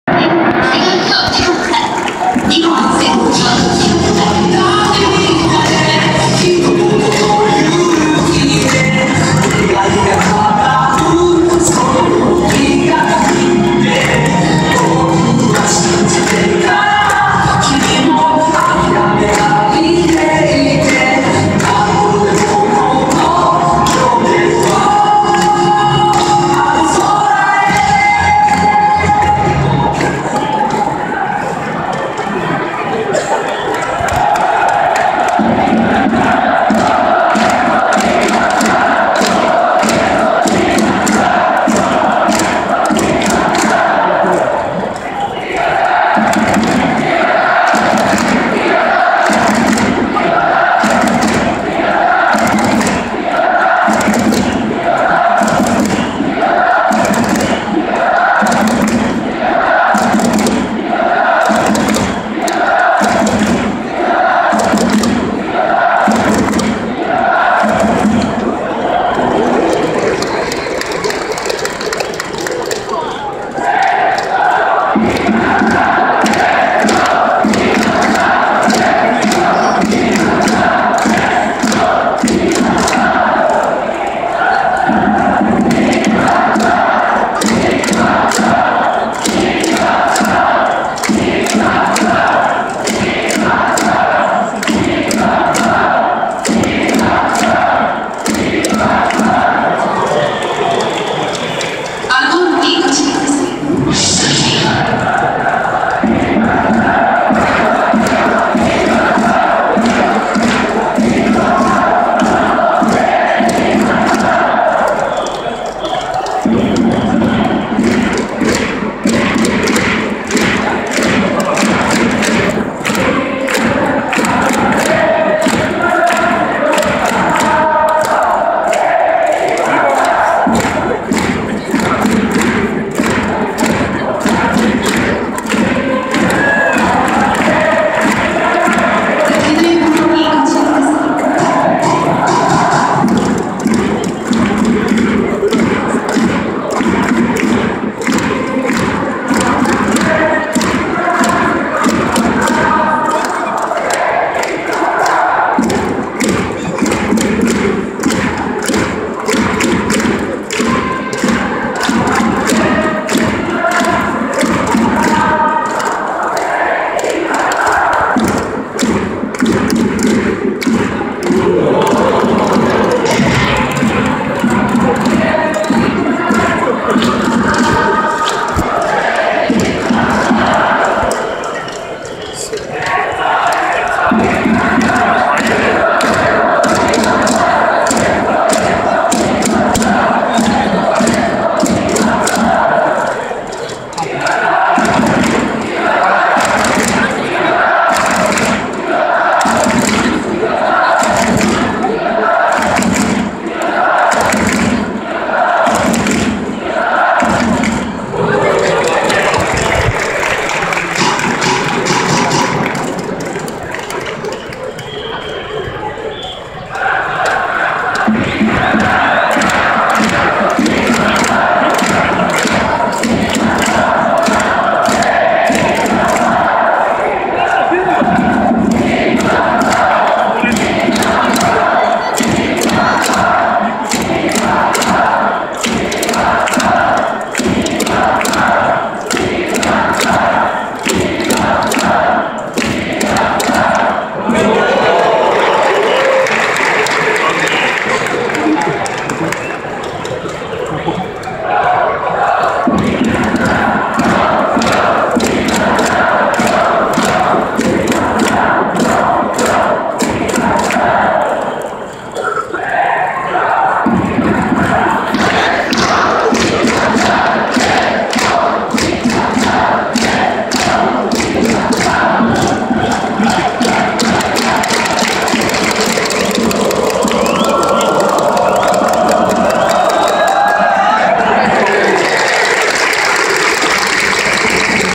March 27, 2015 - Yomiuri Giants Hirokazu Ibata fan cheering chant @ Tokyo Dome
The following is the opening day, game 1 between the Yomiuri Giants vs. the DeNA Yokohama Baystars at Tokyo Dome.  The fans cheering for #2 Hirokazu Ibata.  J!-ENT was in Tokyo to cover the Yomiuri Giants first game of the season.